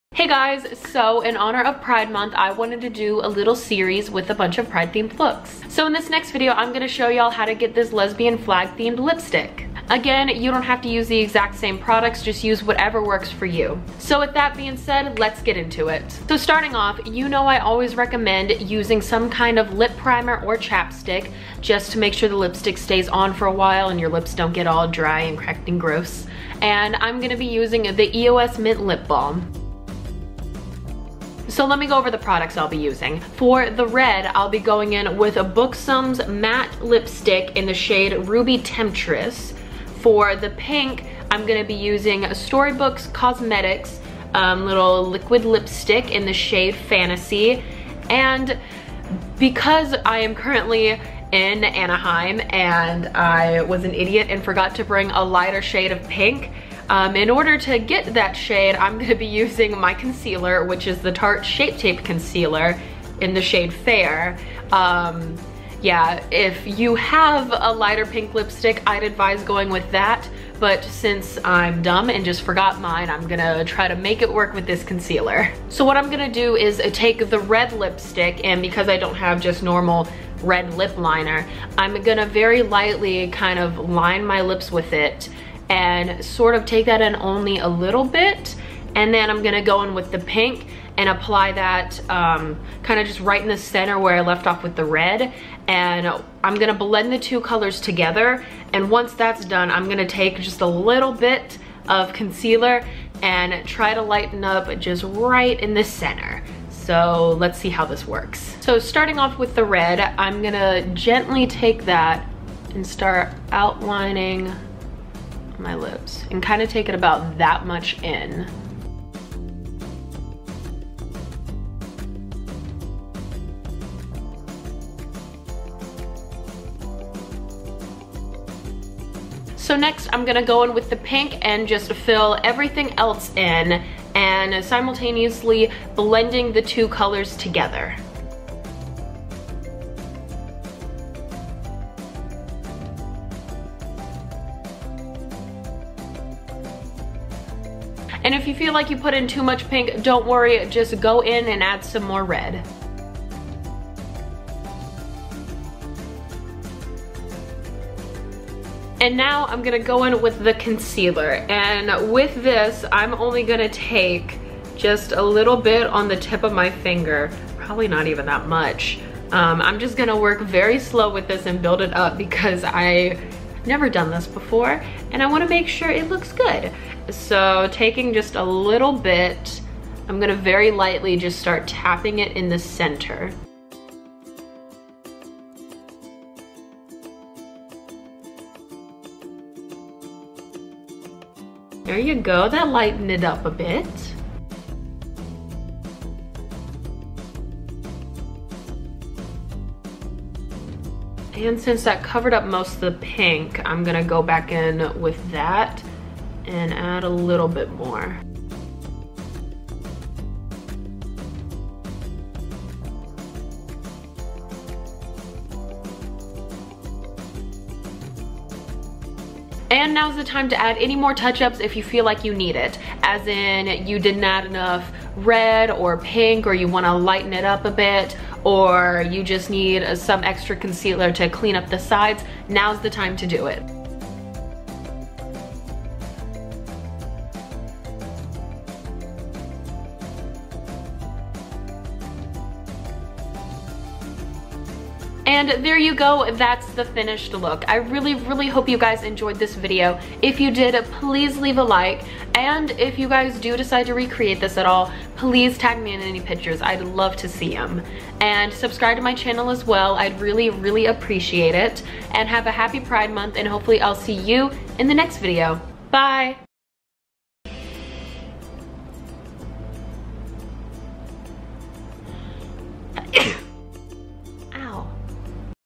Lesbian Themed Lipstick for Pride - Mini Tutorial
Hey guys!! So here's another video in my little Pride series!! I was unsure if I was gonna do this video, mainly because I wasn't sure if the end result would turn out well. And it's not perfect, but I hope you guys like it regardless!

Products used:
Buxom matte lipstick in Ruby Temptress
Storybook Cosmetics liquid lipstick in Fantasy